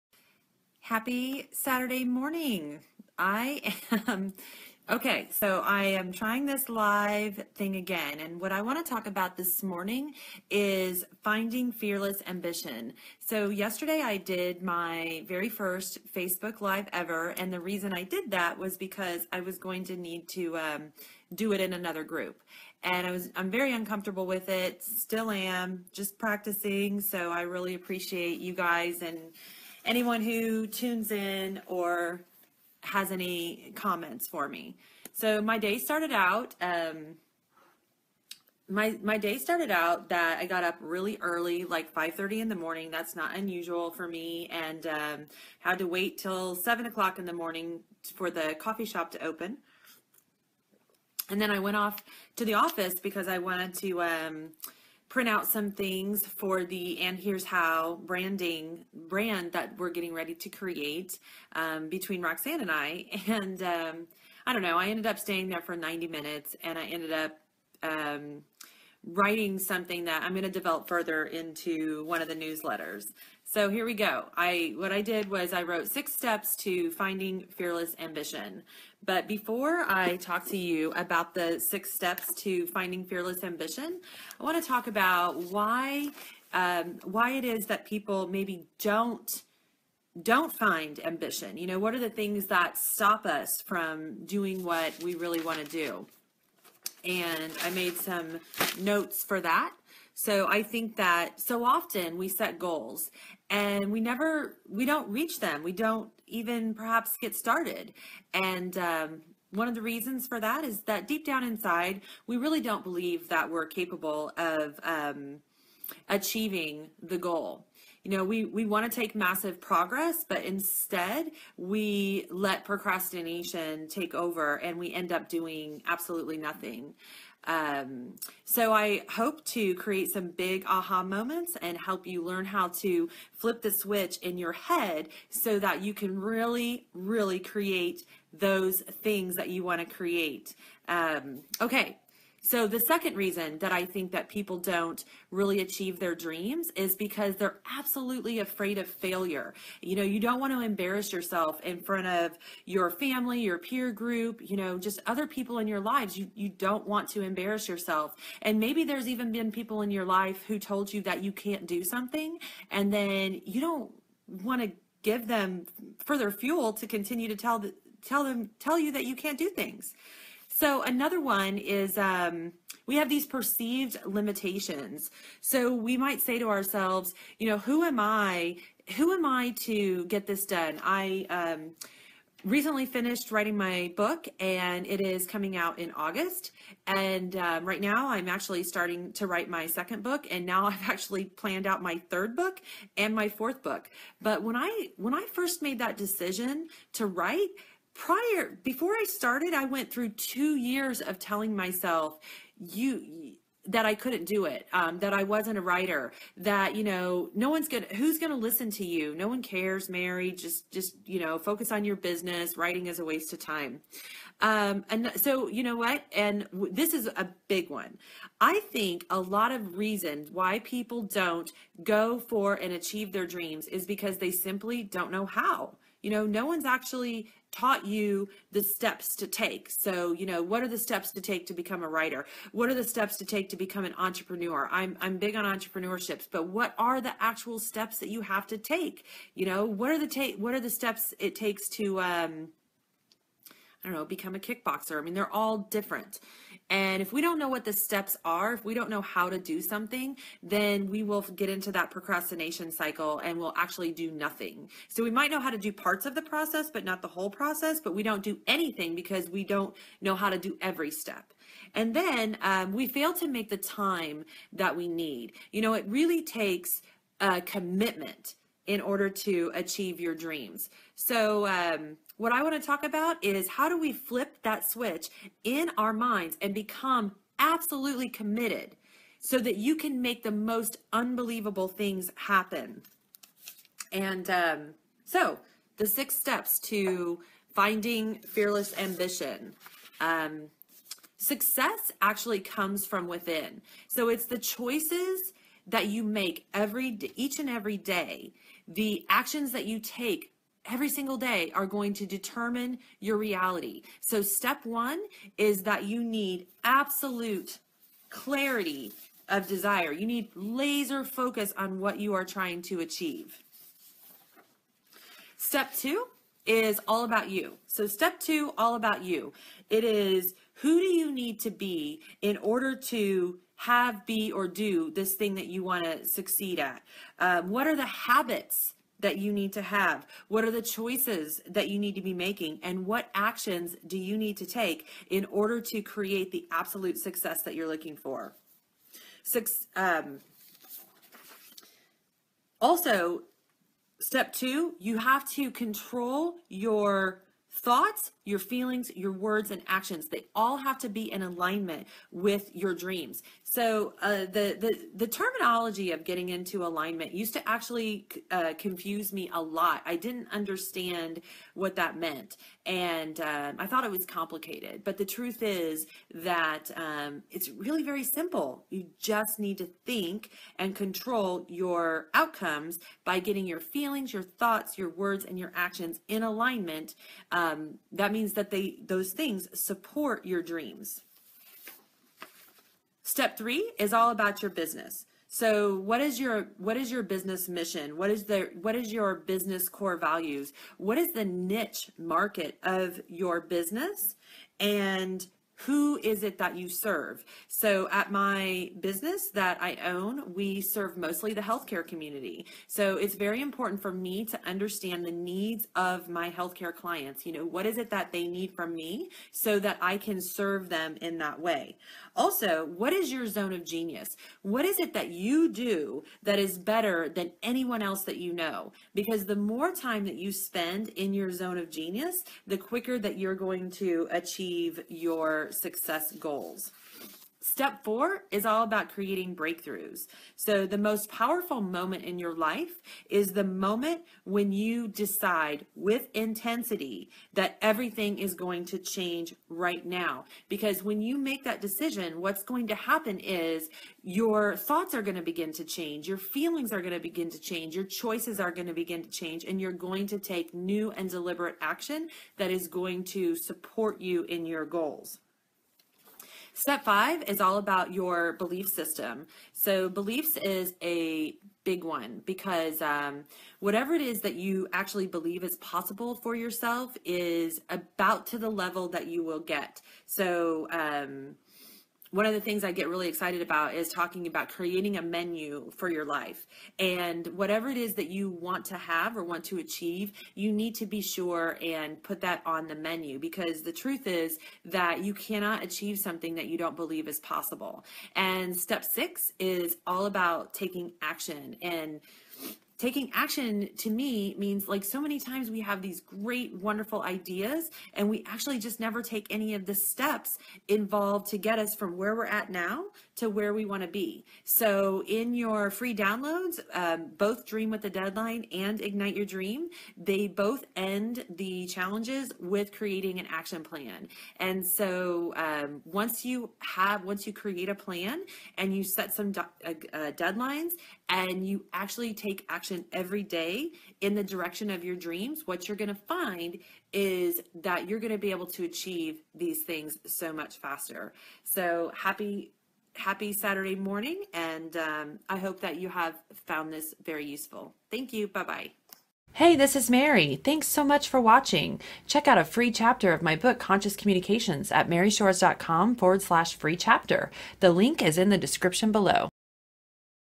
Finding Fearless Ambition
Let's talk about finding that Fearless Ambition in all of us.

#1 Amazon Bestseller in Self-Help, Happiness, and Communication!

Purchase Conscious Communications at your favorite book store or order on Amazon